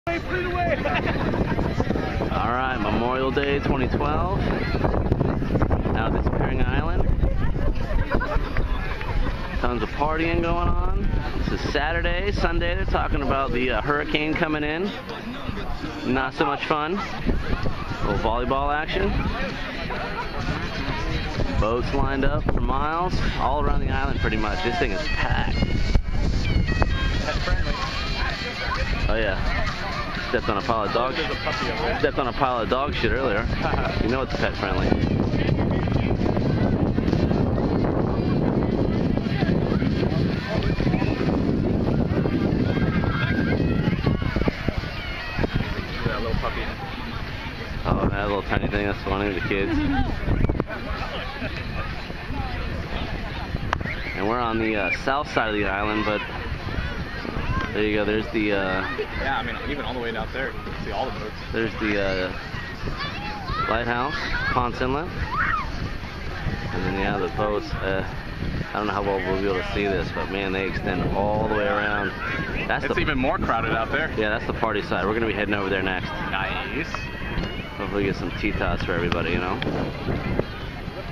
Dissapearing Island Memorial Day 2012
This Memorial day we headed out to Dissapearing Island at Ponce Inlet.  Check my other videos to see the place we rented on the intercoastal.